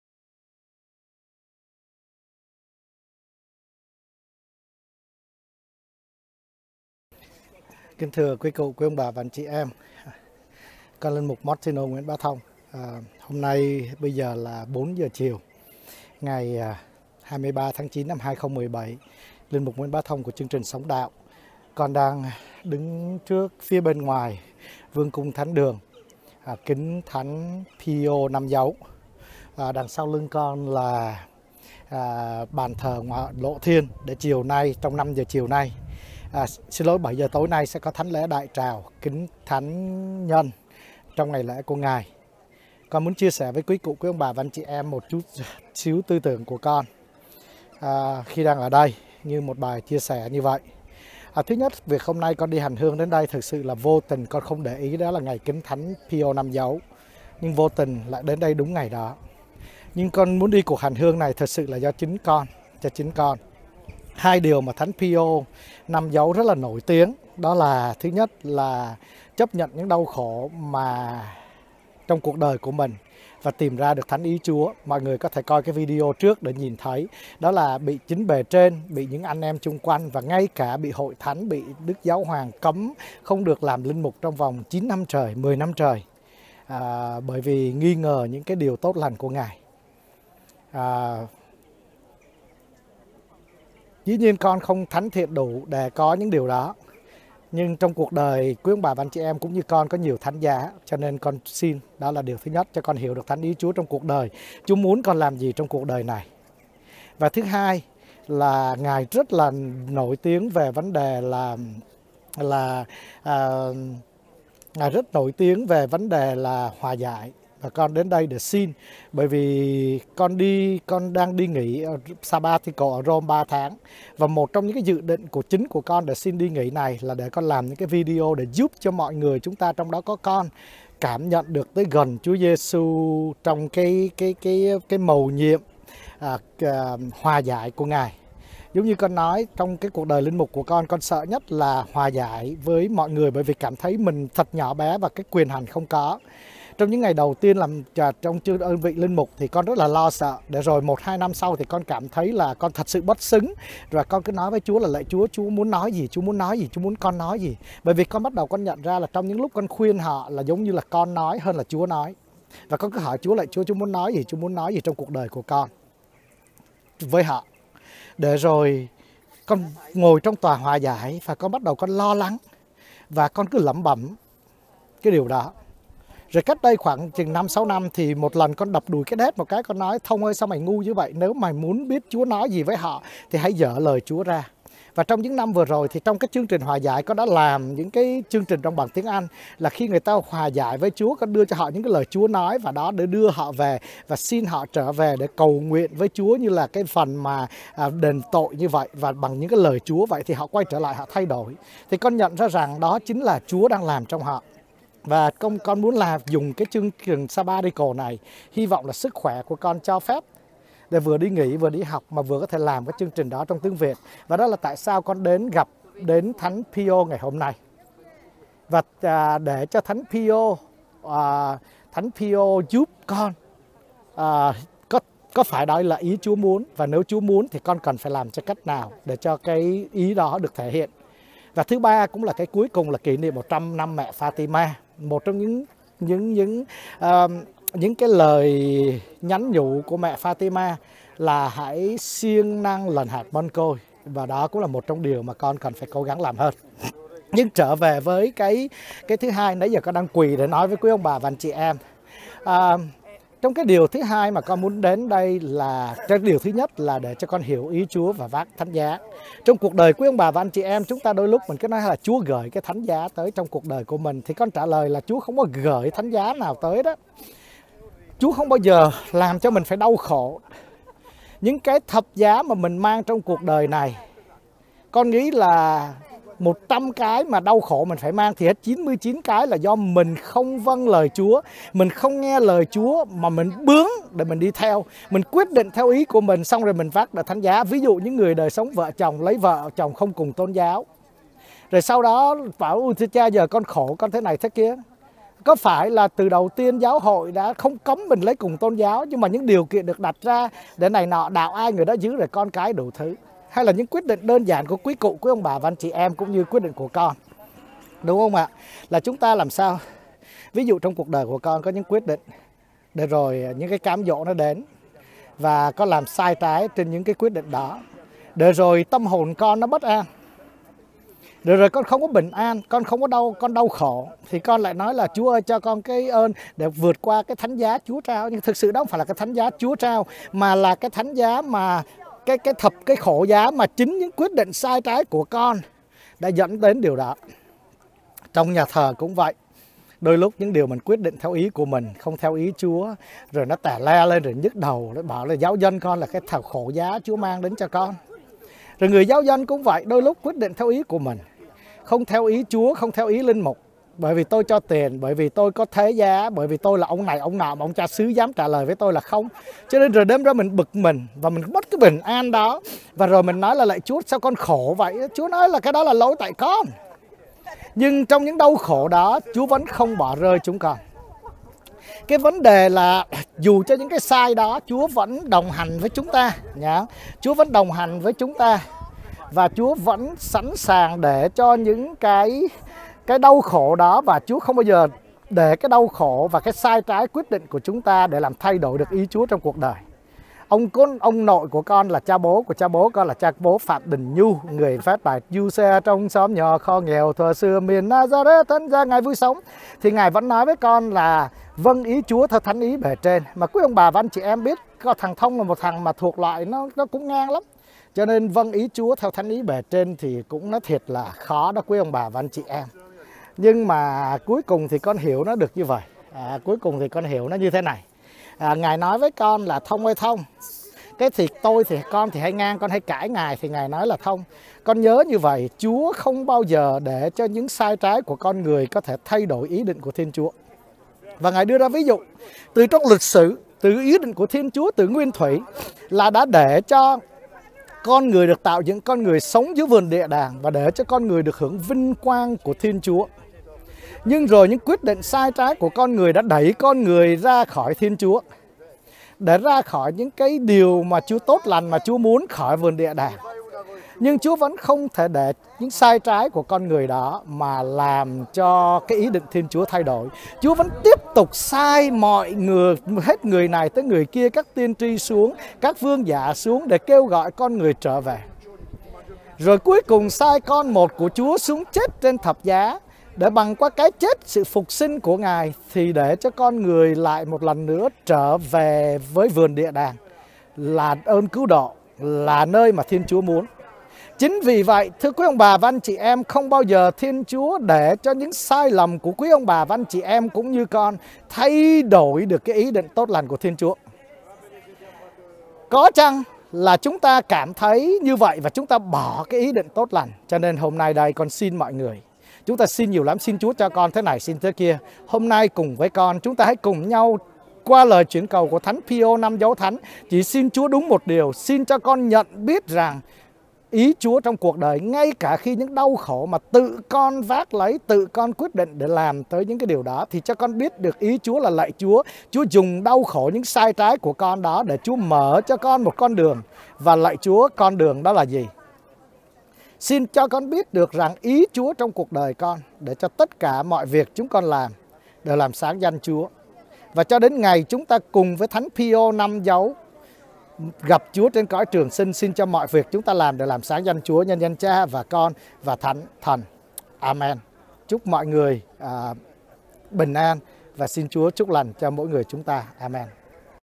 Thánh Pio Năm Dấu
Bài chia sẻ này được quay tại Nhà thờ Kính thánh Pio Năm Dấu thánh  đúng ngày lễ kính Ngài - 23 tháng 9 năm 2017!!
Lm Martino Nguyễn Bá Thông - Chương Trình Sống Đạo

Chủ đề:  xin cho con cùng vác với Ngài!